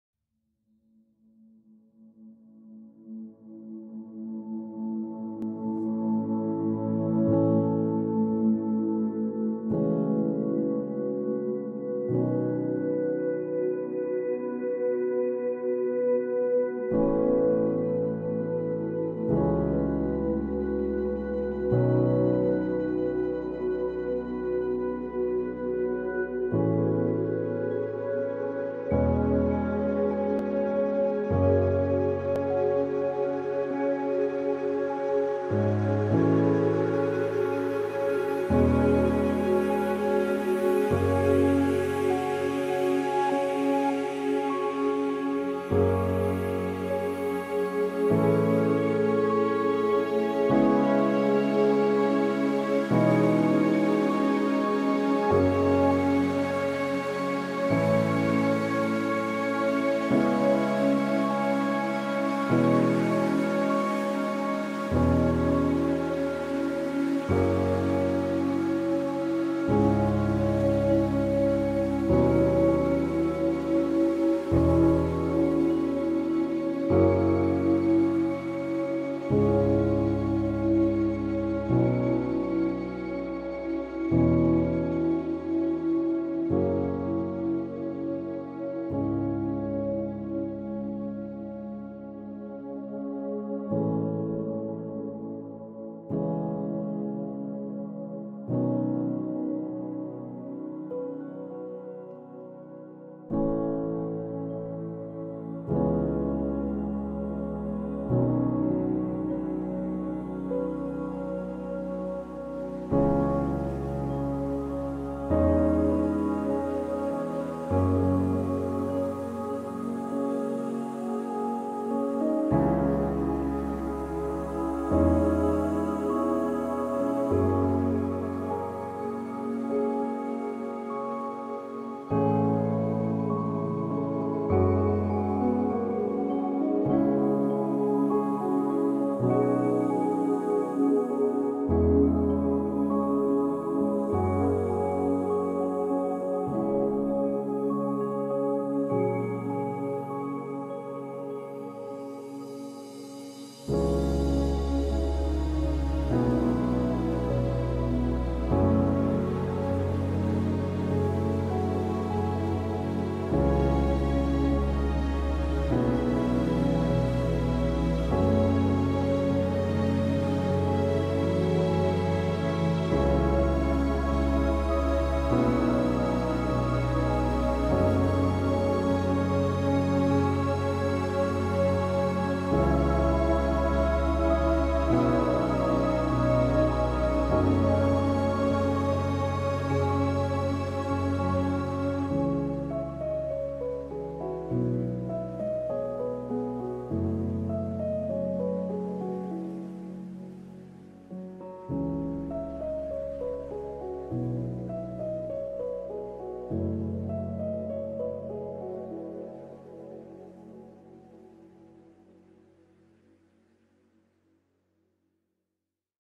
Ethereal Background Music For Videos & Presentation Free(No Copyright)
💿Track description:
Wonderful ambient cinematic music by Punch Deck.